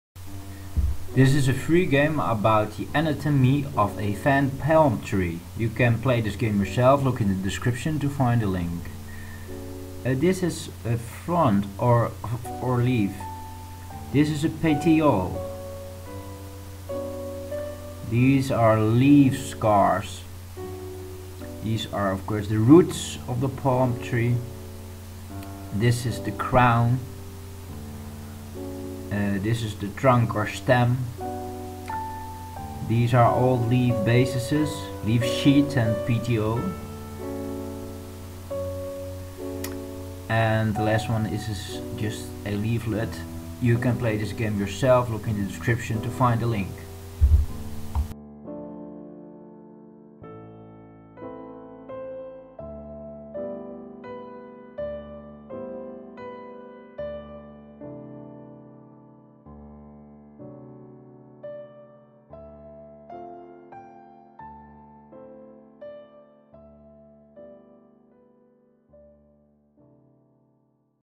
Anatomy of a Fan Palm Tree, trunk ,petiole, crown,leaflet, trunk, stem, leaf scars
costapalmate leaves
roots
trunk or stem
frond or leaf
leaf scars
petiole
leaflet
crown
old leaf bases (leaf sheath and petiole)

Tags Anatomy, science, leaf, Old, leaves, stem, roots, fan, crown, Palm